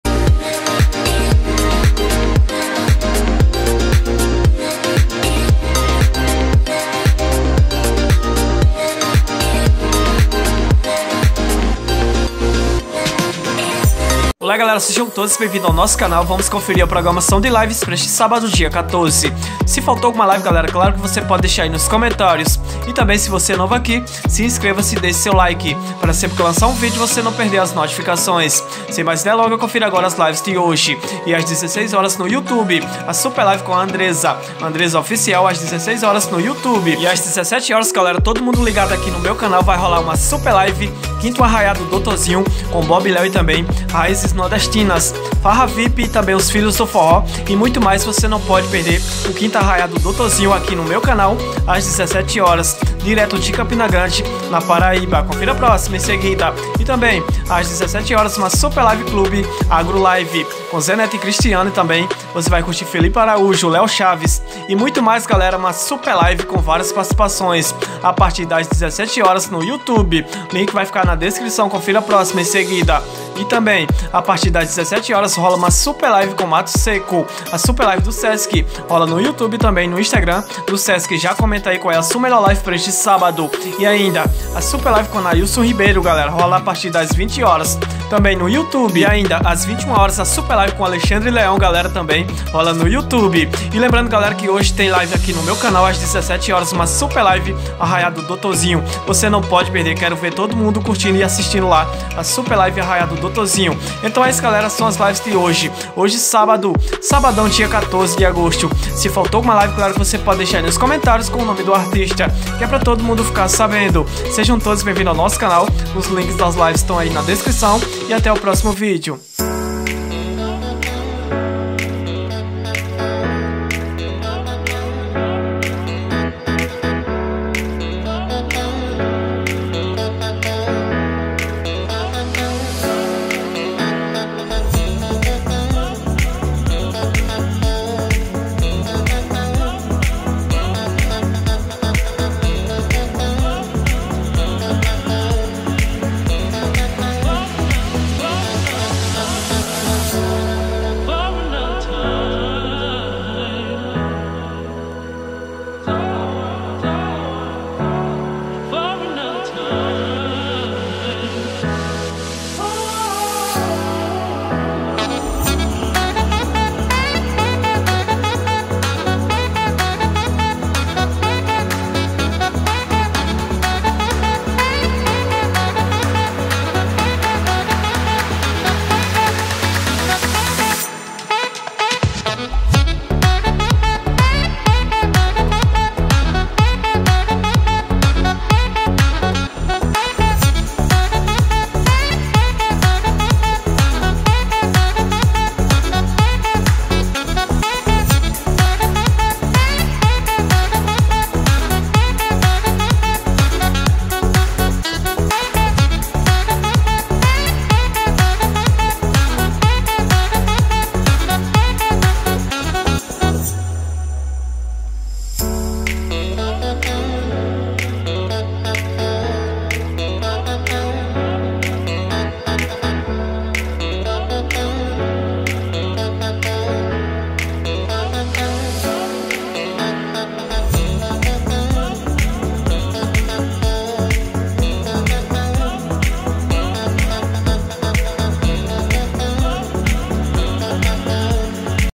LIVES DE HOJE SÁBADO 14/08/2021 - LIVES AO VIVO AGORA | LIVE DE HOJE
Lives de Hoje Sábado 14/08/2021 - LIVE DE HOJE SÁBADO

Quer saber sobre as lives de Hoje?  então vc achou o vídeo certo, para você ficar atualizado, sobre às lives de Hoje,Lives do final da semana!

Lives de Hoje Sábado

✔ Live ARRAIÁ DO DRZINHO 5 com Bob Léo,Forró farra vip,Raízes nordestinas, os Filhos do Forró,Levy Ferreira e muito mais às 17h (aqui no meu canal)

Live Clube Agro Live com Zé Neto e Cristiano,Léo Chaves,João Bosco e Vinícius, Edson e Hudson e César menotti e Fabiano e muito mais | LIVE Clube fm Rio Preto às 17h

Essas são as lives de Hoje Sábado 14/08

Lives do final de Semana

Live clube com Edson e Hudson